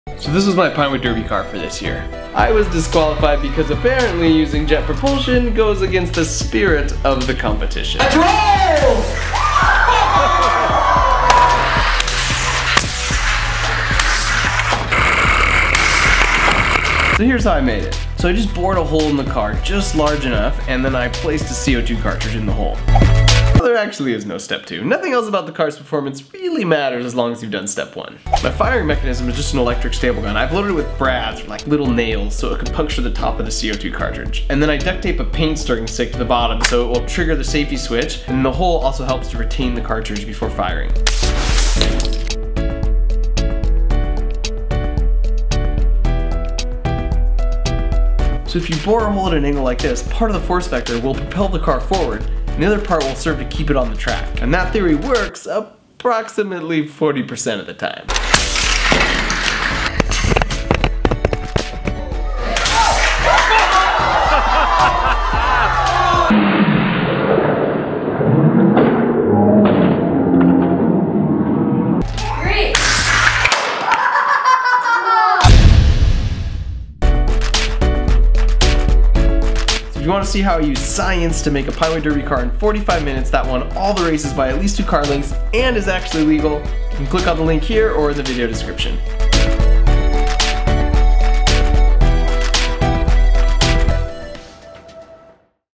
40 MPH Pinewood Derby Car- HOW TO
How to build a 40 MPH (slightly illegal) Pinewood Derby car.

If you want to know how to make a car that is actually legal that still dominates the competition, here y'are